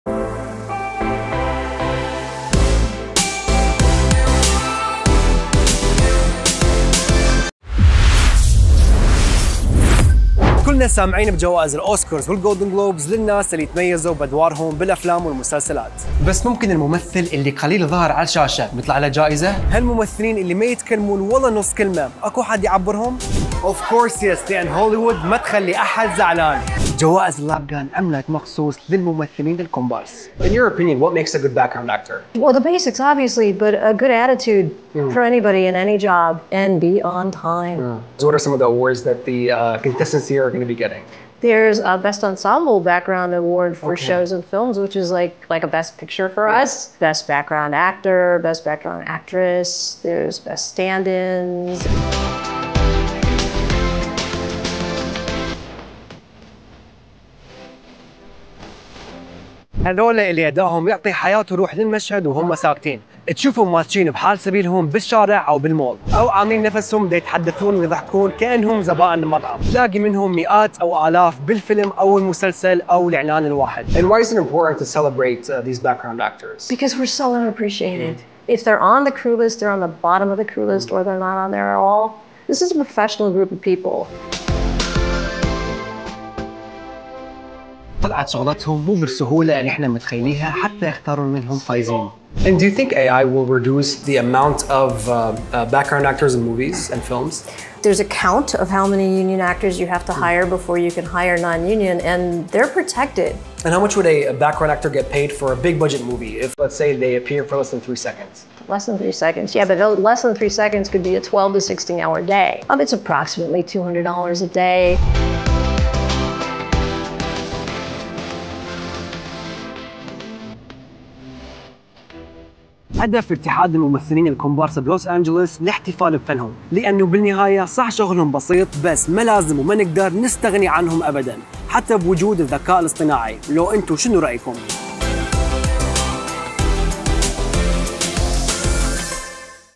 سوبر غريب | أغرب جوائز هوليوود.. جائزة أفضل كومبارس!
في هوليوود جوائز أيضا للكومبارس.. بين تنظيم أفضل فرقة والقدرة على احترام الوقت, تعرفوا على أغرب الجوائر المقدمة للأدوار الثابتة والممثلين الإضافيين في حلقة سوبر غريب الجديدة.
تابعوا بلينكس على جميع المنصات